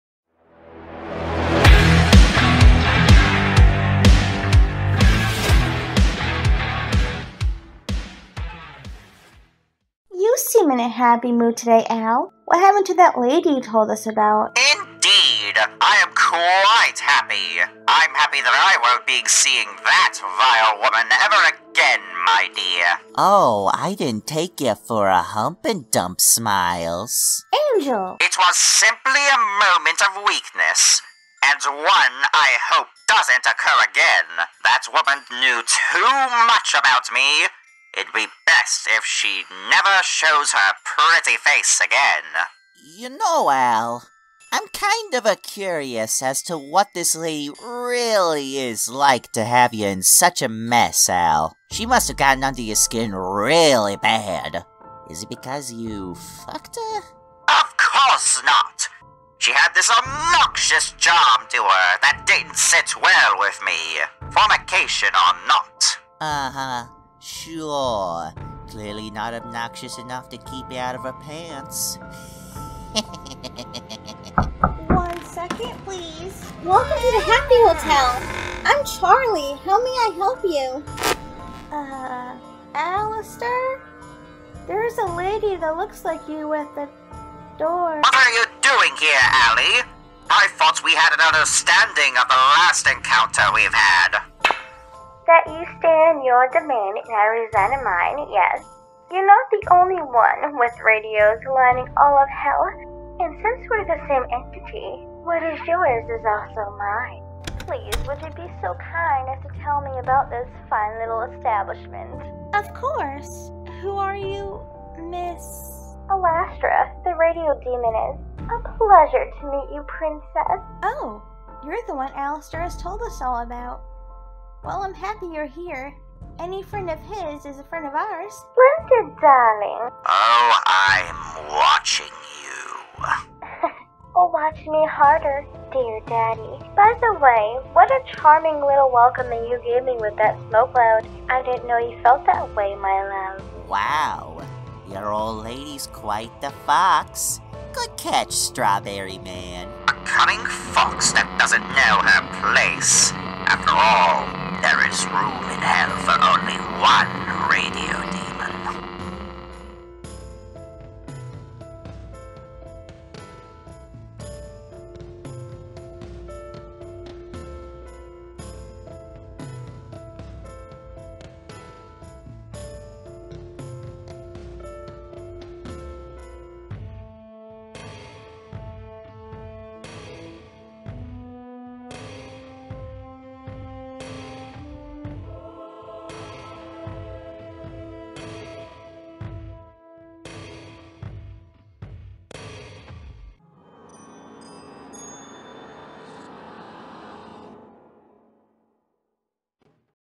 [ALASTRA DUBS] A Match Made In Hell Part 3 - An Unhealthy Obsession [Hazbin Hotel AU Comic Dub]
FINALLY! A Match Made In Hell: Chapter 3 - An Unhealthy Obsession is HERE! We apologize for our little delay on the release today, we were busy with our spooky day. HAPPY HALLOWEEN EVERYONE! :D

More dubs are on the way!. S̸͚͍̟̓̐̐T̵̺̞̼͋͊A̸͚͙̾̕Y̵͍̙͌̿ T̴͔͍͇͑̈́̒U̴͙̙̿̕̕N̴͓͓͇͊͆͝E̸̝͙̻͛̒͝D̴̢͇͕͒́͑

SPECIAL THANKS -

COMIC OWNED BY MARI VA/ALASTRA VA.

Alastra & Charlie - Mari VA/Alastra VA

v  O P E N  F O R  M O R E  v